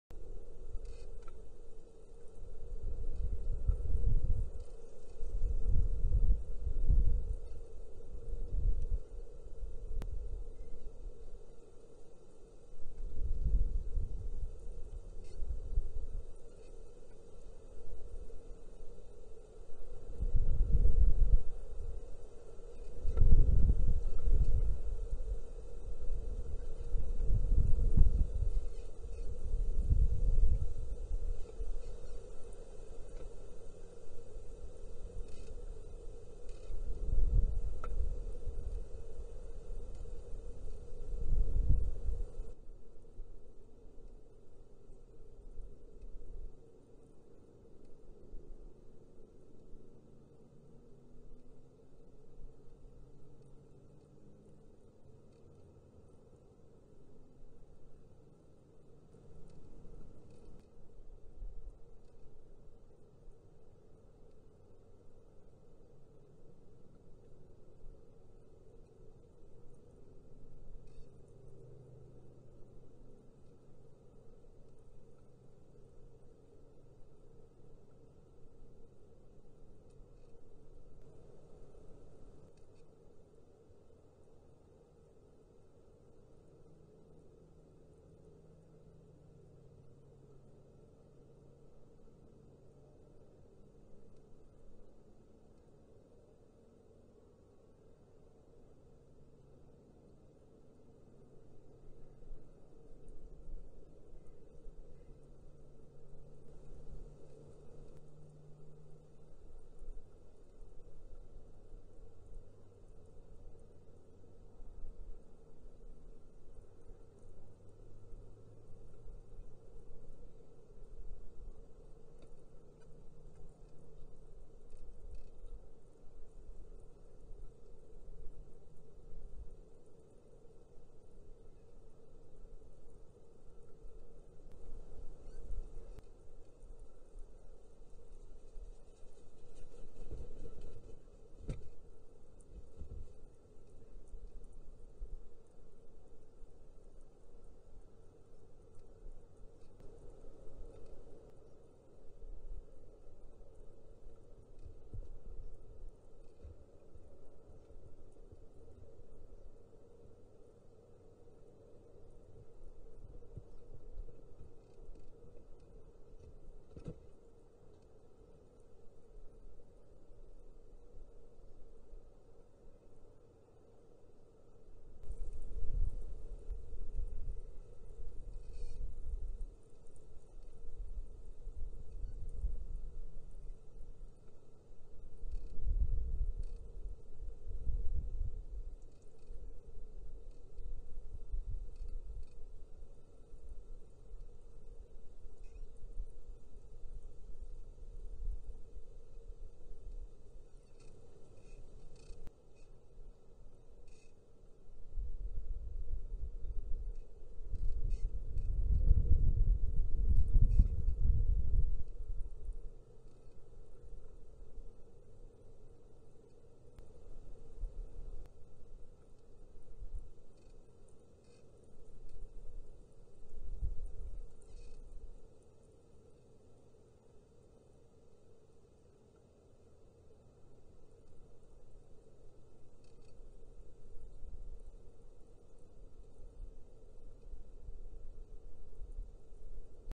30 August WhiteYW back at work in the afternoon/evening ~ FoulshawMoss ©CumbriaWildlifeTrust
©CumbriaWildlifeTrust
FEMALE BLUE 35 - Hatched 2010 - Kielder 1 Nest - Unringed parents
MALE WHITE YW - Hatched 2008 - Bassenthwaite - Unringed parents